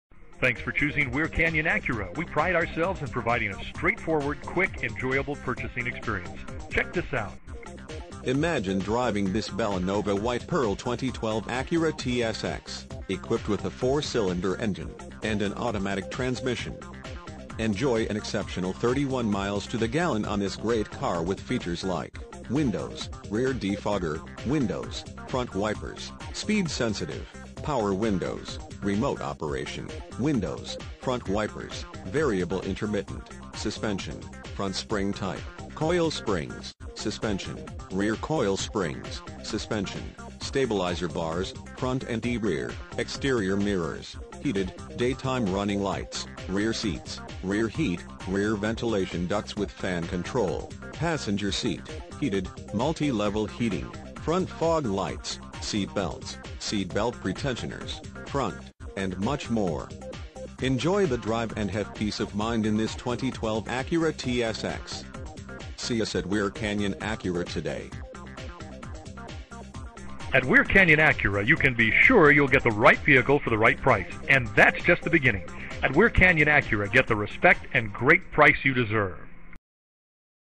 2012 Acura TSX Weir Canyon Acura Anaheim, CA 92807
Acura TSX Imagine driving this Bellanova White Pearl Acura Tsx 4 Dr Sedan, equipped with a 4 Cylinder engine and an automatic transmission.
MILES: 31,240
VIN:JH4CU2F40CC012951

8323 E La Palma
Anaheim, California 92807